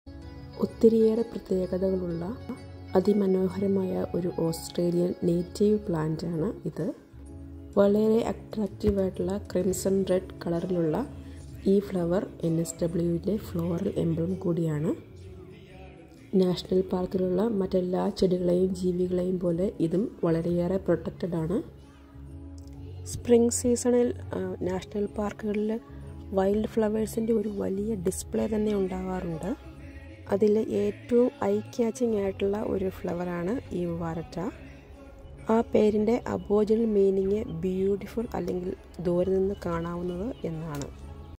Waratah/ NSW Floral emblem shorts
Waratah a crimson red very attractive very beautiful native plant of Australia.  it's also NSW floral emblem.  It's very commonly seen in national parks and sandstone ridges. It's a protected species and Incurs heavy fines if caught picking it.
The word Waratah means beautiful or seen from afar.